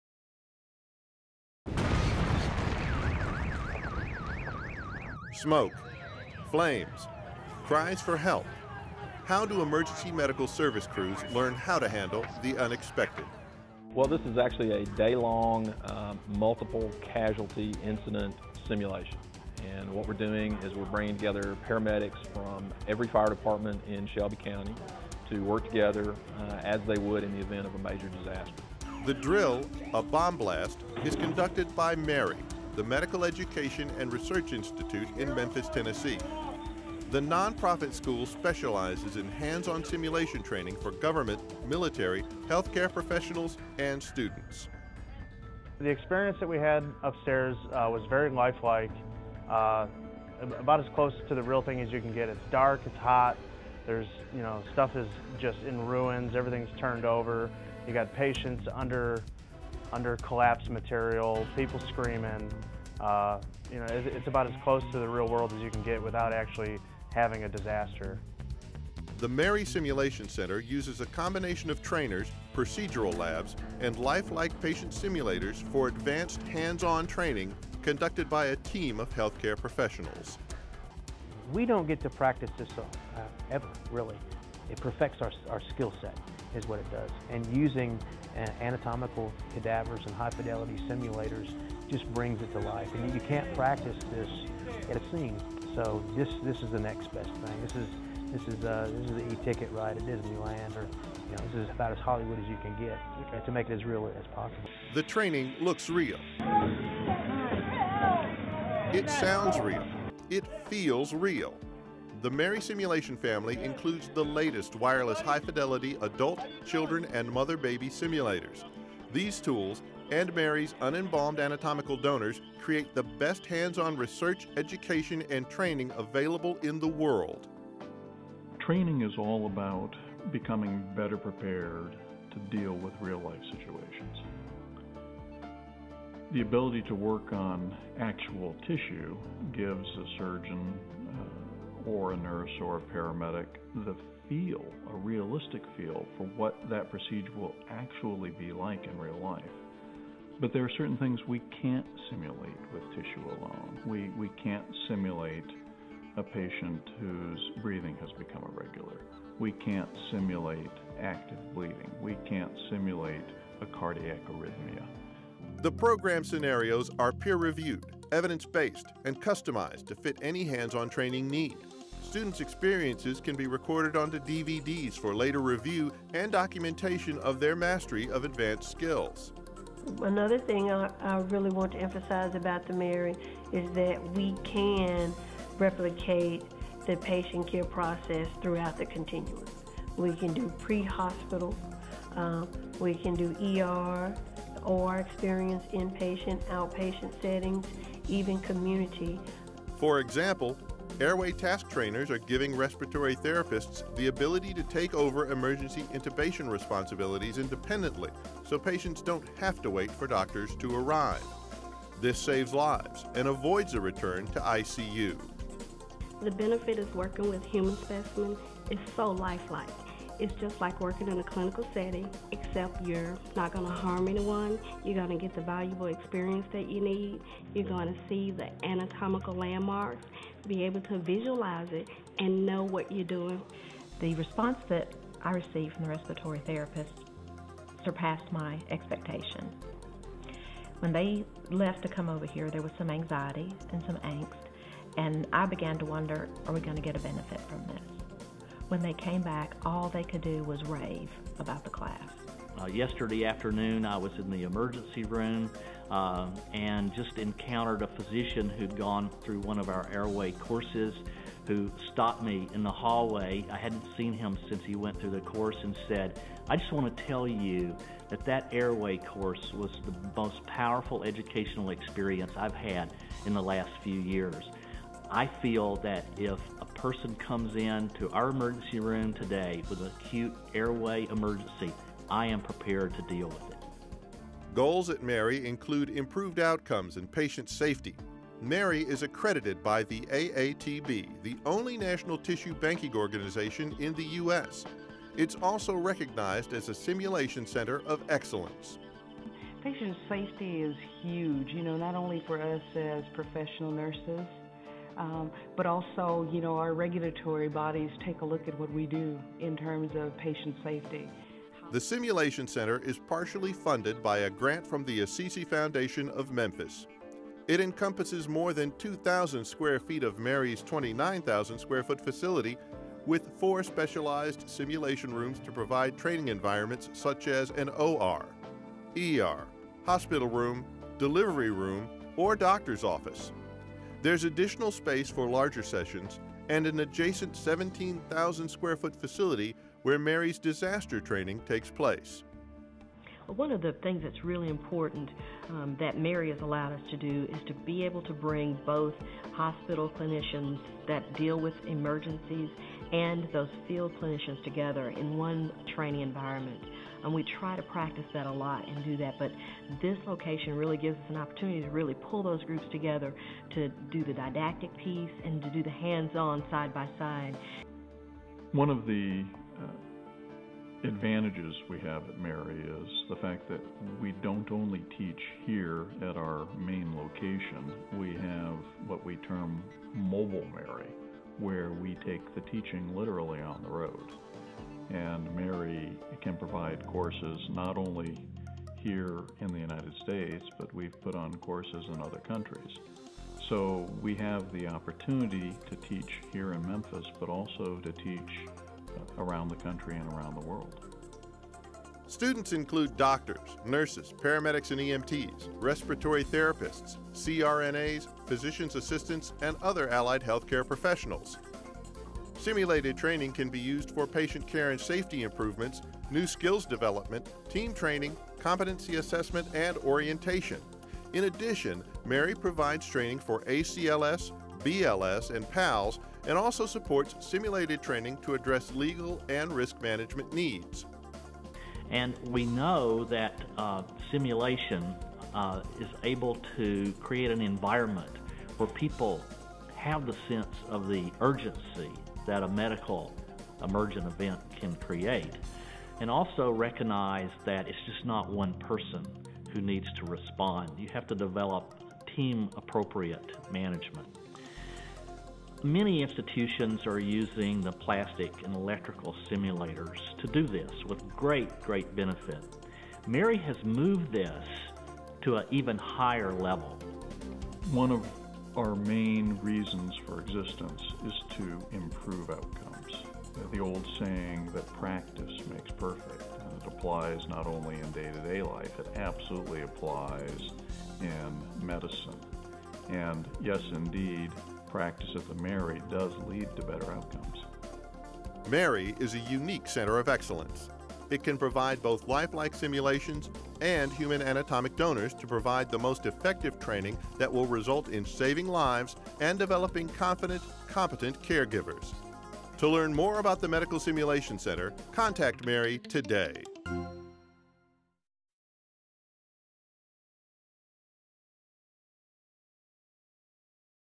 MERI Simulation 2011.mp4
The MERI Simulation Center of Excellence provides hands-on medical education training and research to enhance patient safety, which results in saving lives and better patient outcomes. The MERI simulations create the best-practice hands-on training environment to develop and demonstrate individual and team care skills in all areas of medical care, emergency and disaster training. MERI's Simulation Center of Excellence is partially funded by a donation from the Assisi Foundation of Memphis.